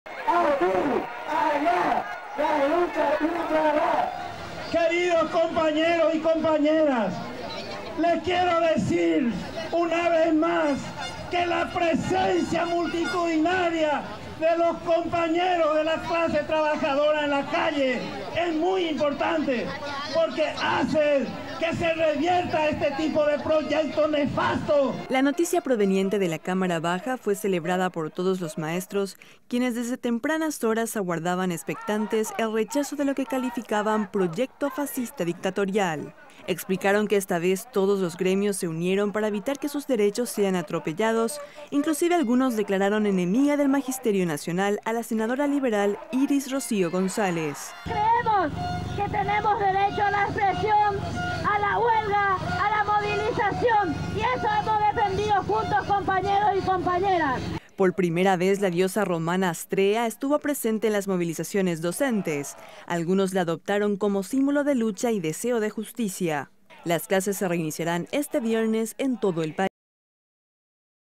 TERMINO LA HUELGA DOCENTE
TRAS ENTERARSE DE QUE LA CÁMARA DE DIPUTADOS RECHAZÓ EL PROYECTO DE LEY QUE MODIFICA EL CALENDARIO ESCOLAR, LOS DOCENTES DE DISTINTOS GREMIOS CONGREGADOS EN LA PLAZA UBICADA FRENTE AL CONGRESO, FESTEJARON LA NOTICIA.